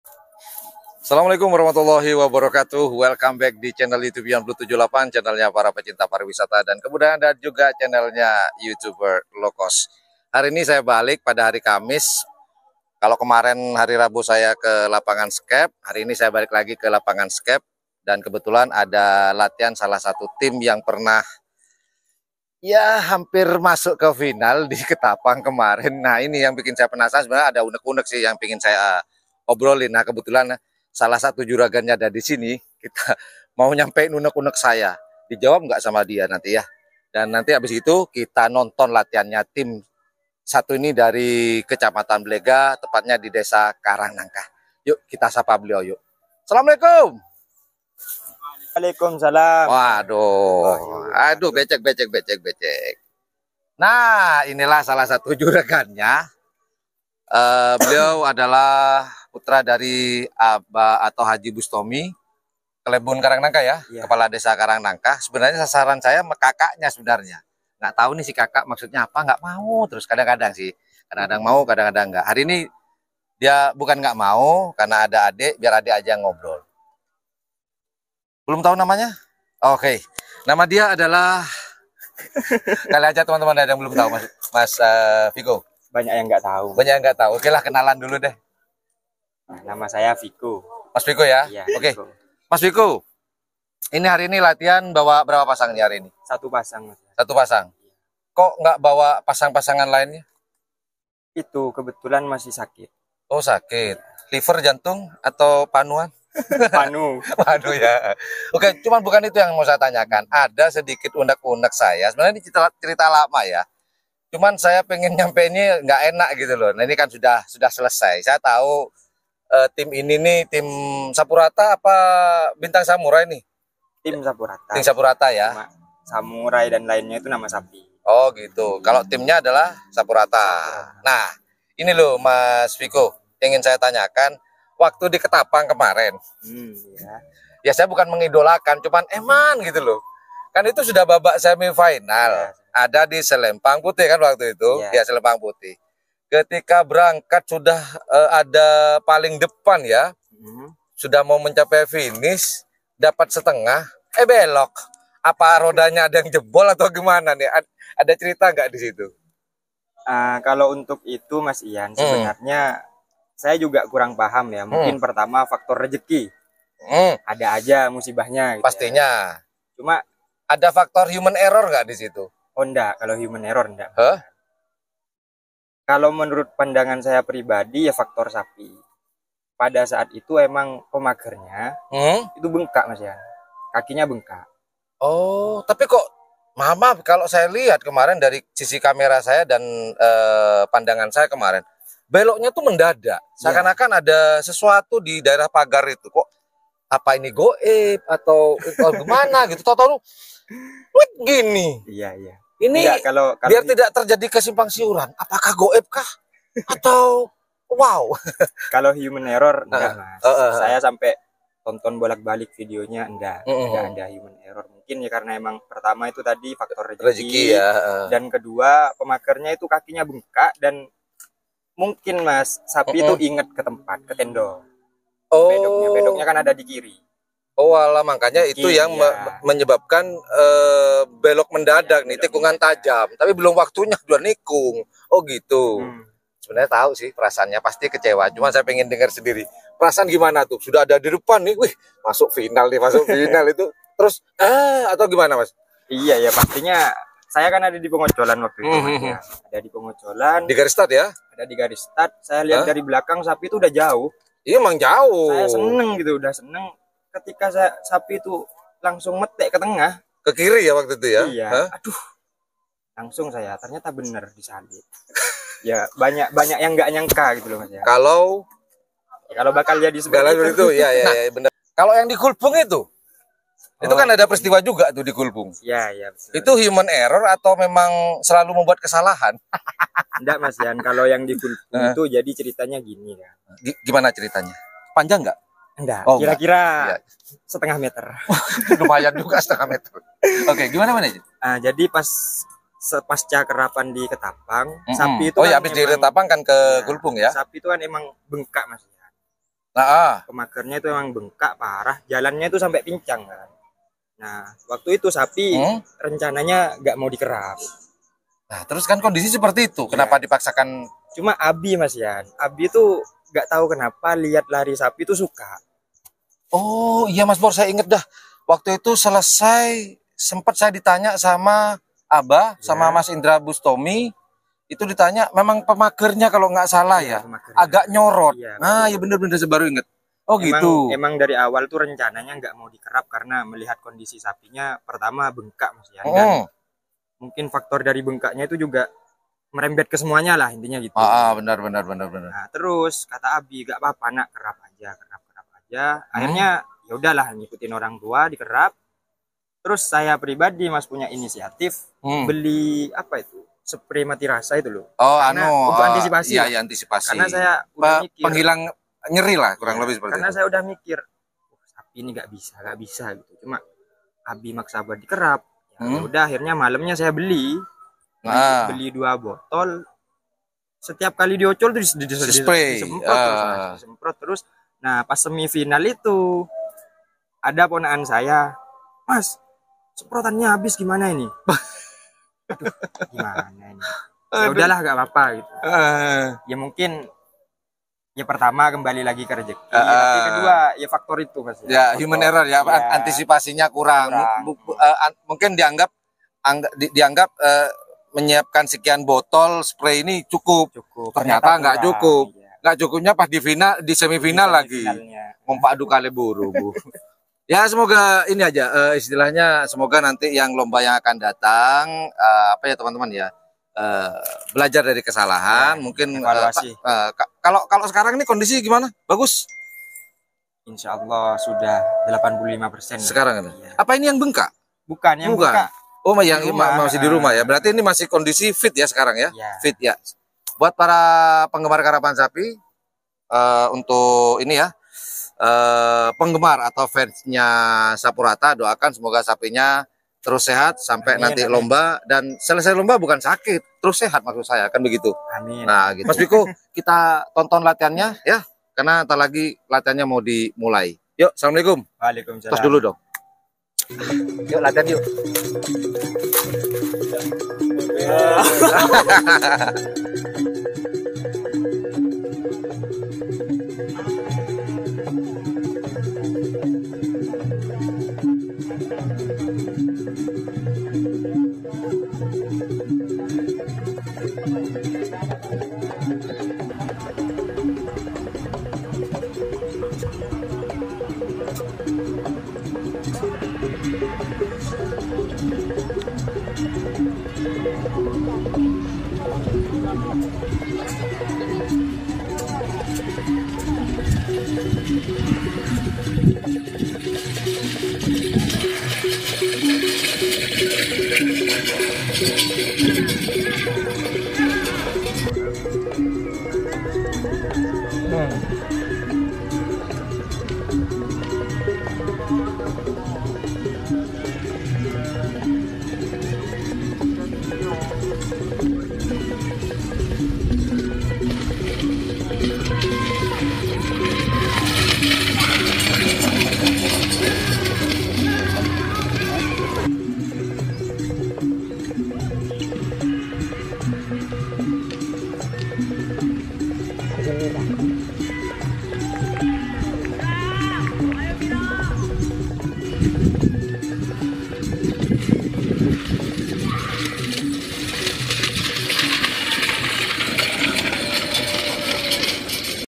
APES 2 KALI | Ngobrol Santai & Latihan Tim Karapa Sapi SAPURATA
sembari latihan saya coba tanyak tanyak berkenaan dengan ke apesan atau kesialan tim Karapan Sapi Sapurata ini besutan H.Bustomo kepala desa Karang Nangka Blega.